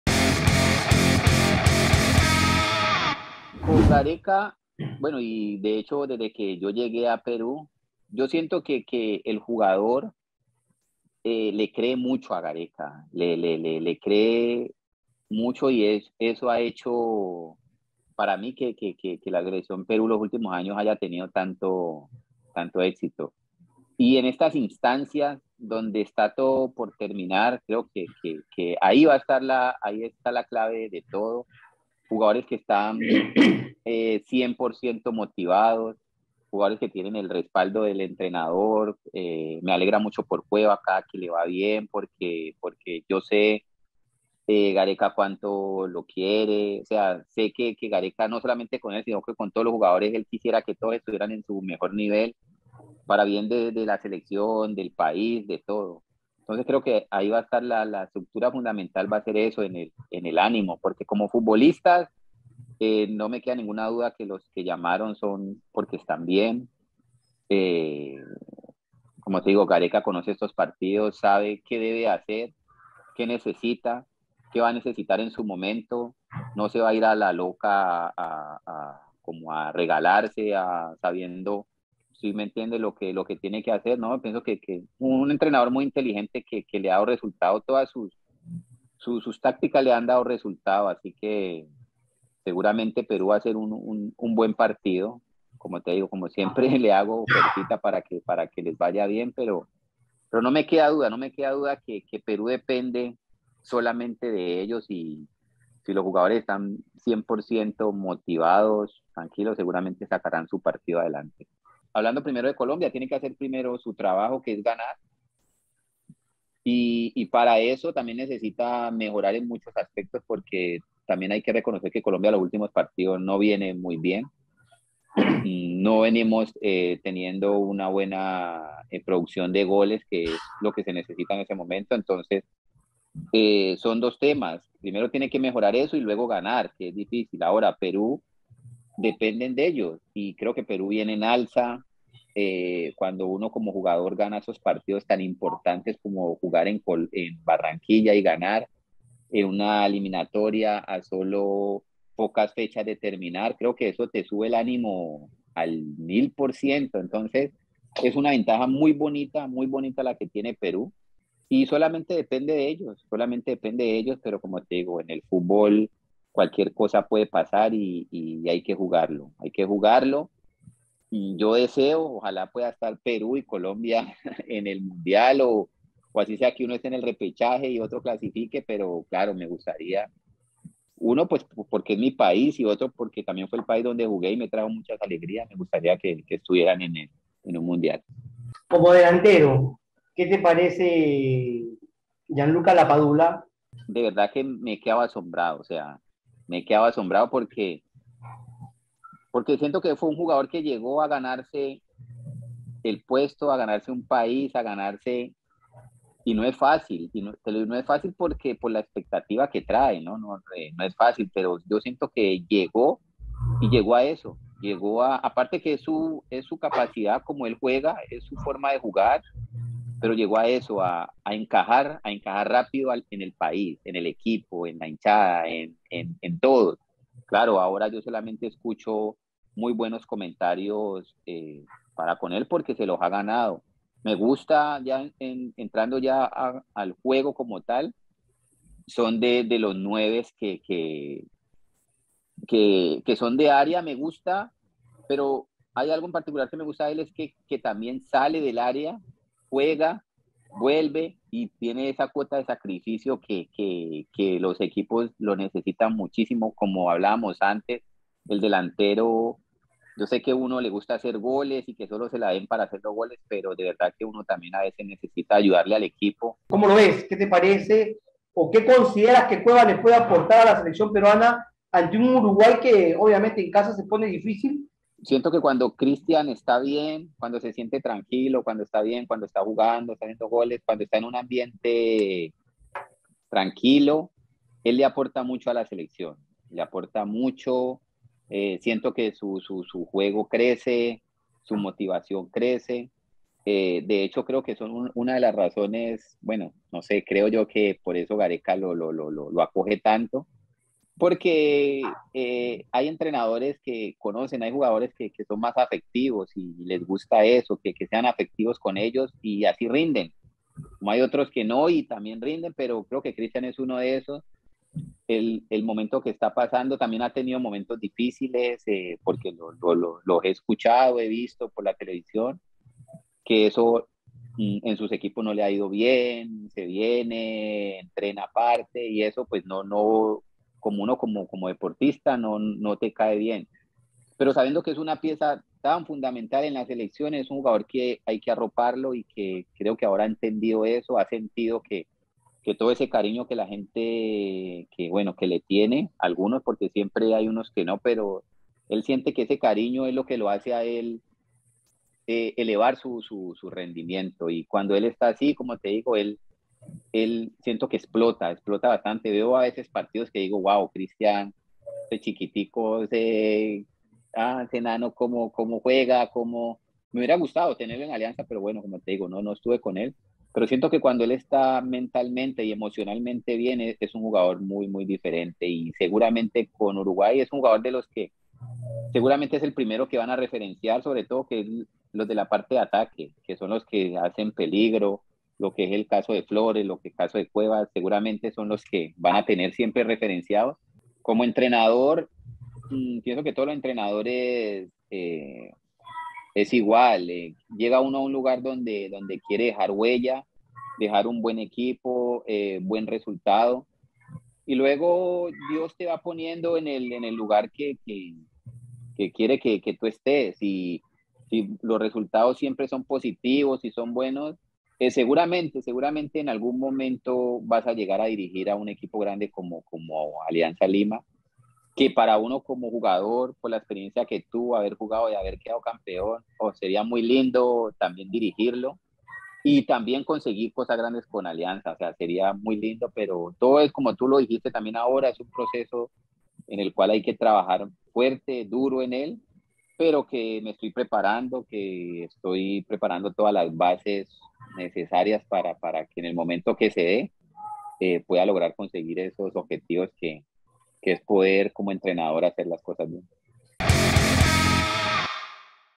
"EL JUGADOR PERUANO LE CREE MUCHO A GARECA" || Lionard Pajoy analiza a la Blanquirroja
El ex delantero colombiano Lionard Pajoy, quien logró campeonar con Alianza Lima en 2017, dijo que la confianza de los jugadores al técnico Ricardo Gareca ha hecho que la SelecciónPeruana tenga tanto éxito y les va a servir de motivación cuando enfrente a Uruguay por las Eliminatorias.

Entrevista: Christian Ñiquen